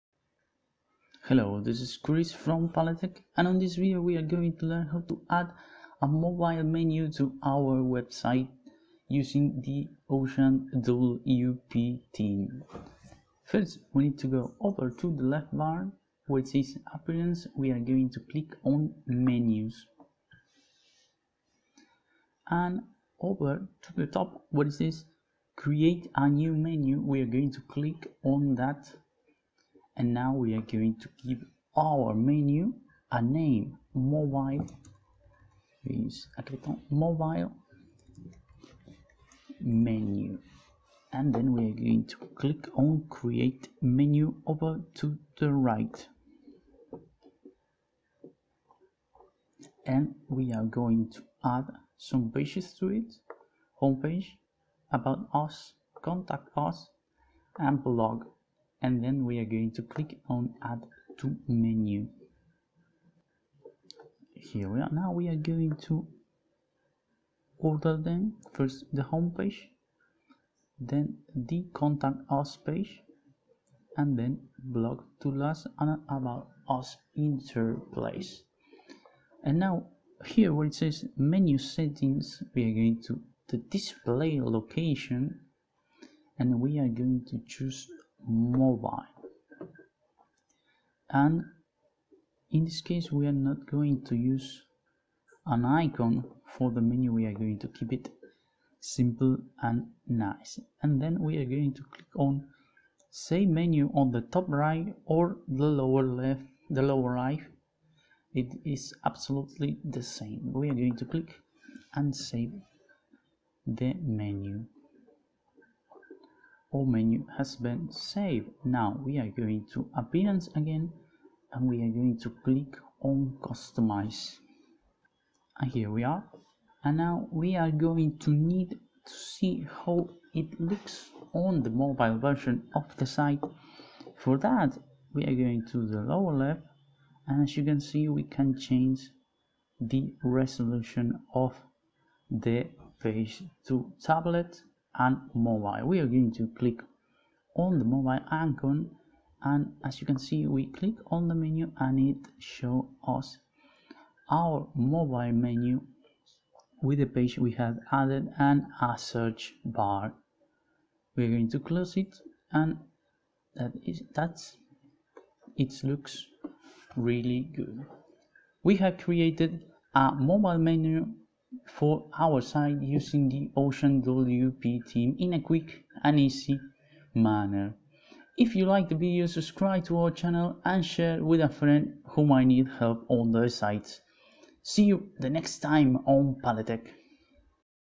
How to add a Mobile Menu to your Wordpress Website
Add a custom Mobile Menu to your Wordpress Website with OceanWP Theme.